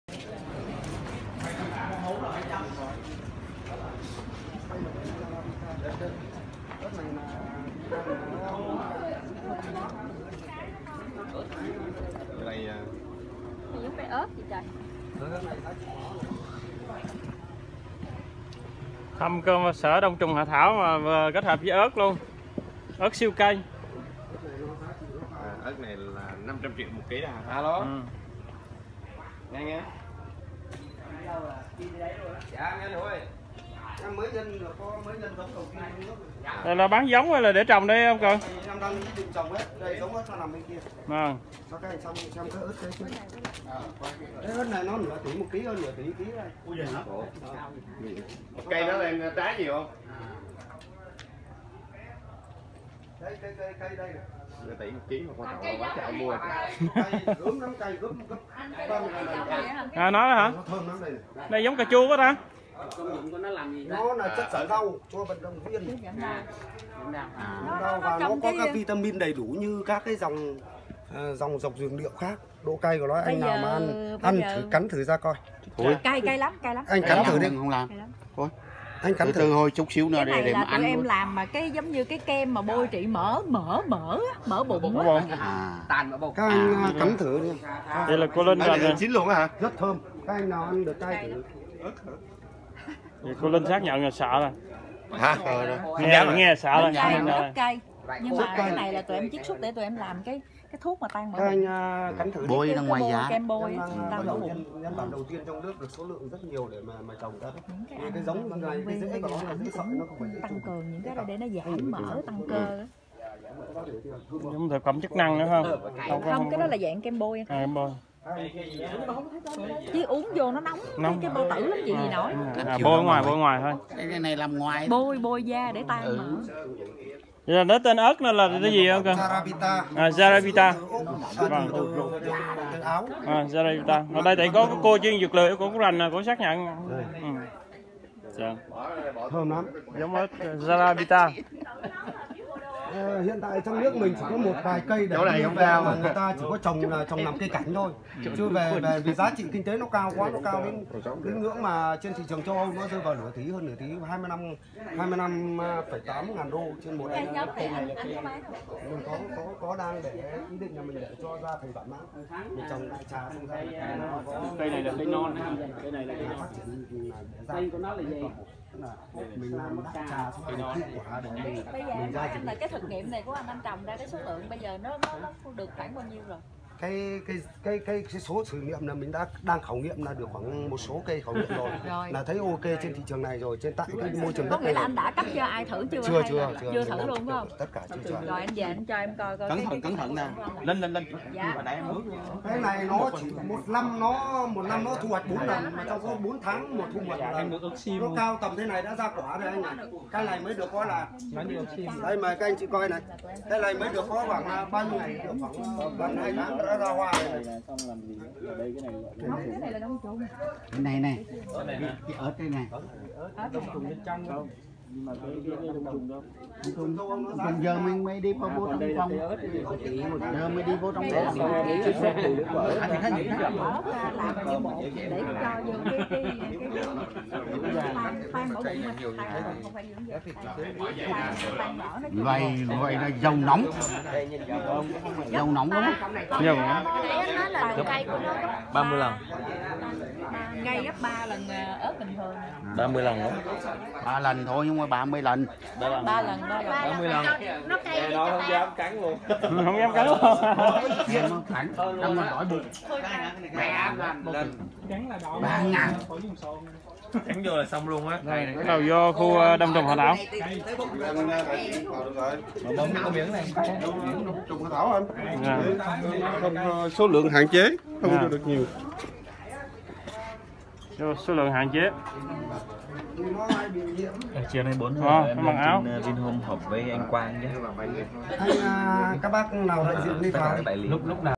Giống ớt siêu cay Charapita giá 0.5 tỷ đồng/kg tại Daknong
Ớt siêu cay giá 500tr đồng/kg, dùng làm dược liệu kem bôi tan mỡ bụng, không dùng để uống hoặc ăn trực tiếp như ớt thông thường. Ớt có tên Charapitam mới du nhập vào VN những năm gần đây.